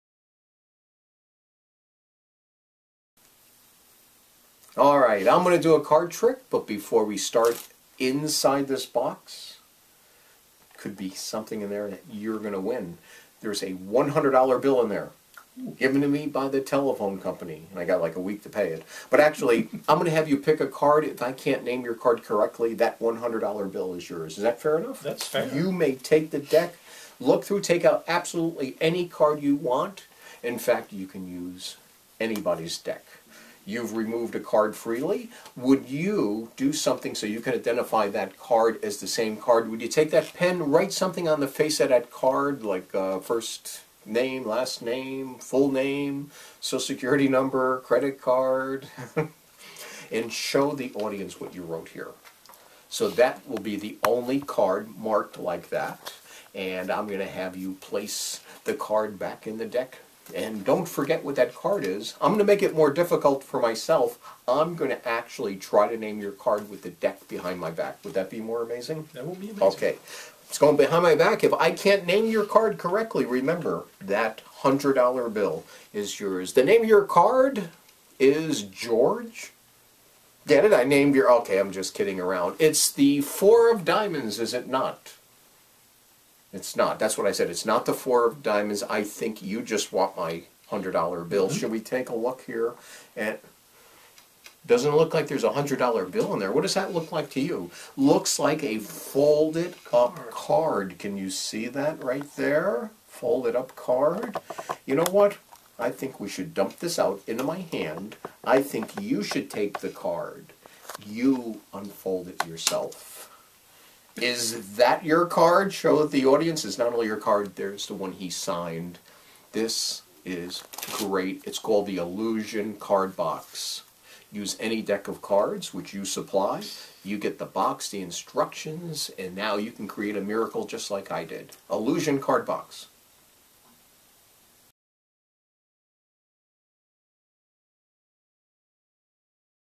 Illusion Card Box Magic Trick
A signed card vanishes from the deck and appears inside a wooden box that has been in full view the whole time. The amazing ILLUSION CARD BOX trick is available